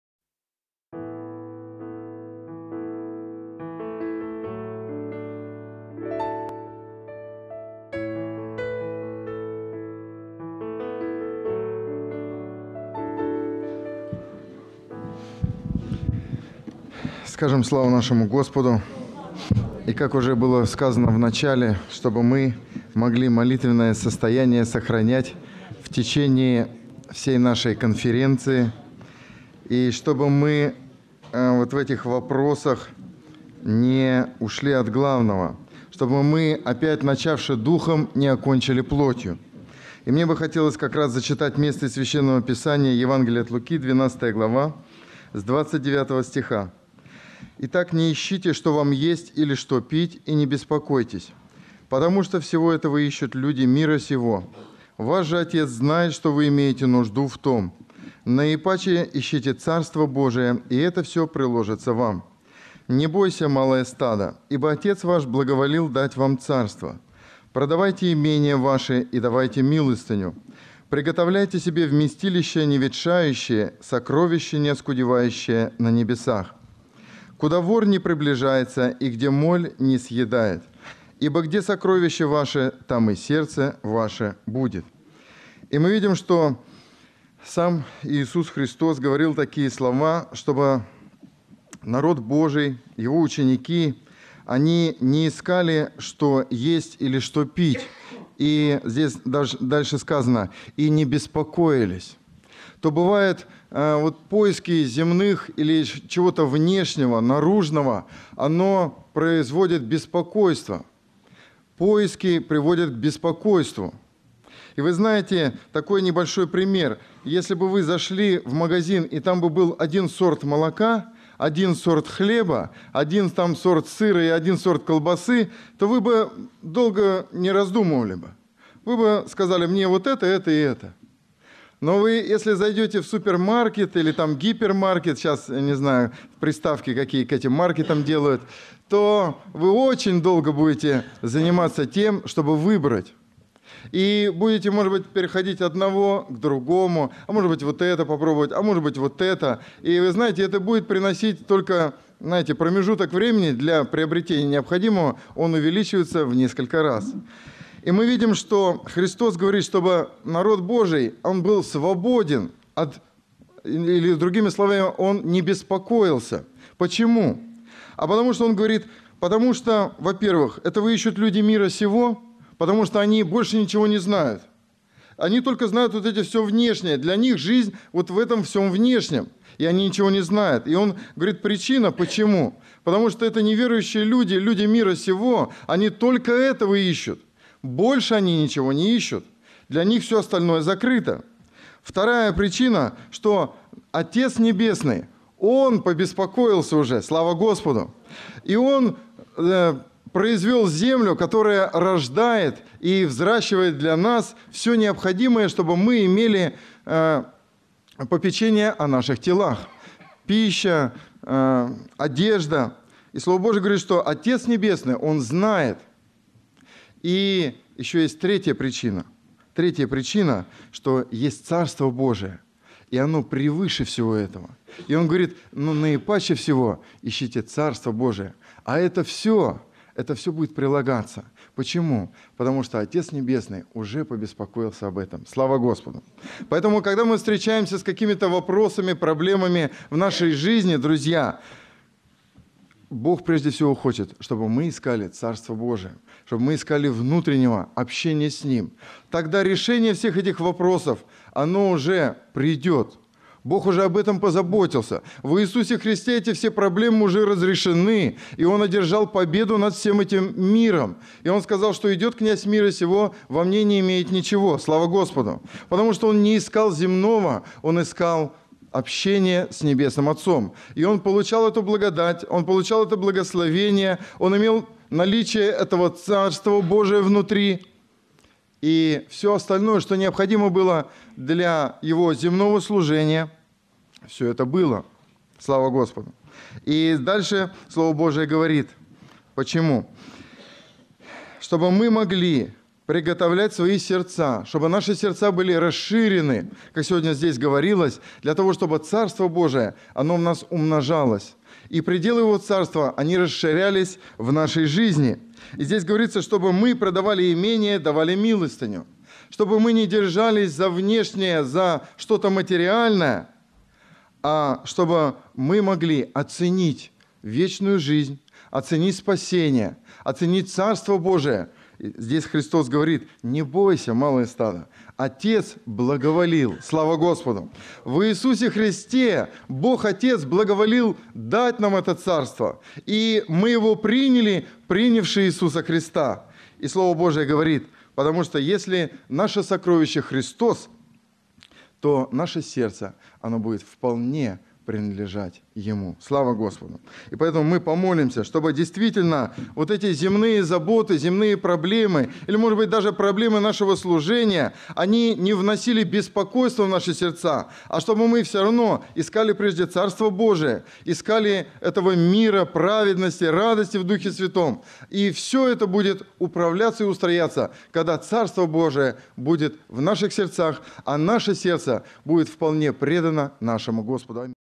16 Олег Белый Ищите Царствия Божия
2007 IX Конференция учителей¹ ВШ² ОЦХВЕ Сибири
¹Учитель - исторически сложившейся термин, обозначающий человека ответственного за организацию мероприятий проводимых для детей, подростков и взрослых прихожан. Не является лицом осуществляющим образовательную или воспитательную деятельность.
²Воскресная школа - исторически сложившейся термин, обозначающий христианскую просветительскую деятельность среди детей, подростков и взрослых прихожан. Не является образовательным учреждением. Название образовано от дня проведения мероприятий - так как обычно они проводятся по воскресеньям.